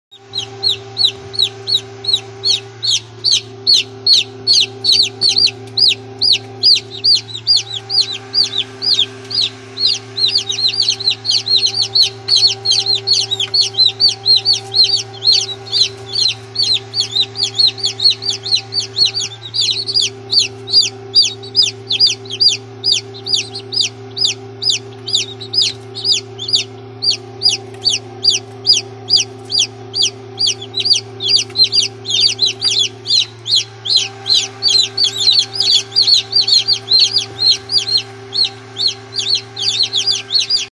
Australorp Chicks Coming Out Of Eggs
Australorp chicks first day in incubator